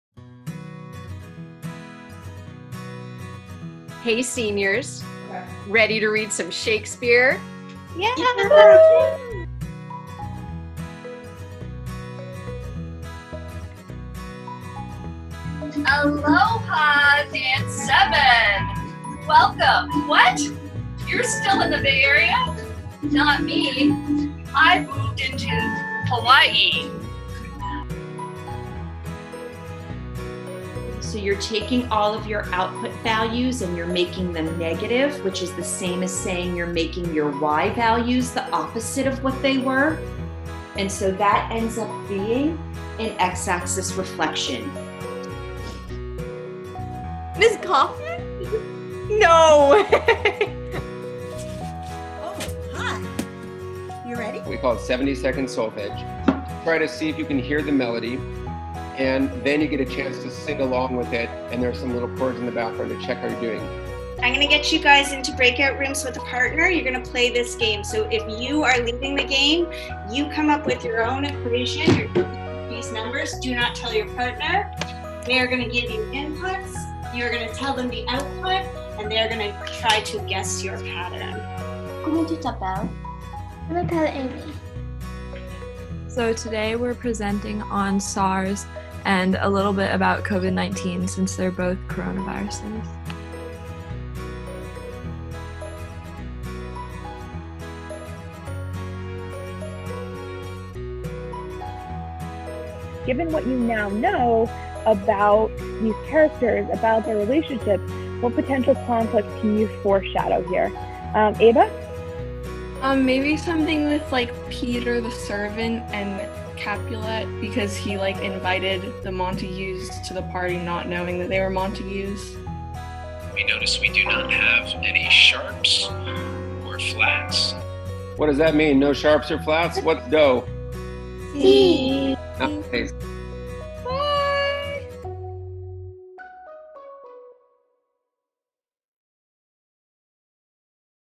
Distance Learning at Castilleja
From learning the hula in dance, exponential functions in math, to solfeges in chorus, distance learning is in full swing at Castilleja!